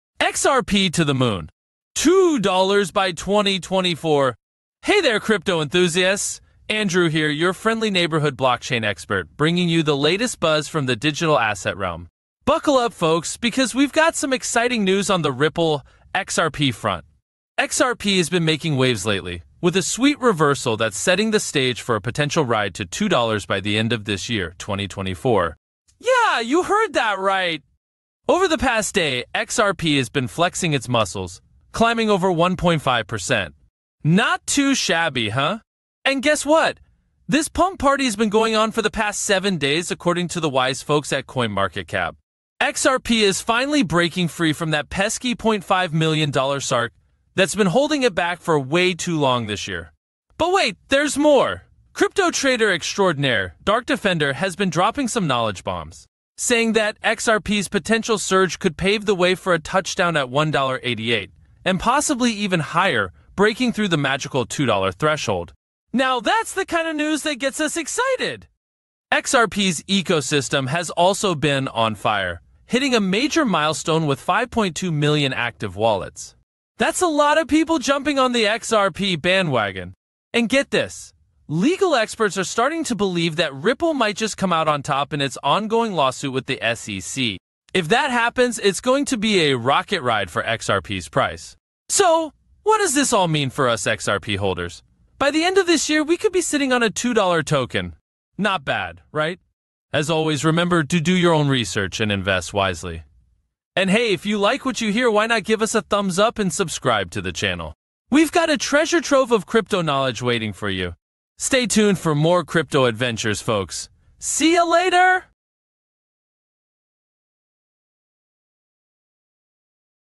🚀 XRP to the Moon: $2 by 2024! 🚀
💰💼 Bybit Copytrading 32+ million successful transactions, 75% with profit Earn money comfortably.

🚀 XRP to the Moon! $2 by 2024? 📈 | Ripple (XRP) Latest News and Analysis

Hey there, crypto enthusiasts! Andrew here, your friendly neighborhood blockchain expert, bringing you the latest buzz from the digital asset realm.

Buckle up, folks, because we've got some exciting news on the Ripple (XRP) front. XRP has been making waves lately, with a sweet reversal that's setting the stage for a potential ride to $2 by the end of this year, 2024. 🚀

Over the past day, XRP has been flexing its muscles, climbing over 1.5%. Not too shabby, huh? And guess what? This pump party has been going on for the past seven days, according to the wise folks at CoinMarketCap. XRP is finally breaking free from that pesky $0.50 mark that's been holding it back for way too long this year. 🤑

But wait, there's more! Crypto trader extraordinaire, Dark Defender, has been dropping some knowledge bombs, saying that XRP's potential surge could pave the way for a touchdown at $1.88, and possibly even higher, breaking through the magical $2 threshold. 🤯 Now, that's the kind of news that gets us excited!

XRP's ecosystem has also been on fire, hitting a major milestone with 5.2 million active wallets. That's a lot of people jumping on the XRP bandwagon! And get this: legal experts are starting to believe that Ripple might just come out on top in its ongoing lawsuit with the SEC. If that happens, it's going to be a rocket ride for XRP's price. 🚀

So, what does this all mean for us XRP holders? By the end of this year, we could be sitting on a $2 token. Not bad, right? 💰

As always, remember to do your own research and invest wisely. We've got a treasure trove of crypto knowledge waiting for you. 📚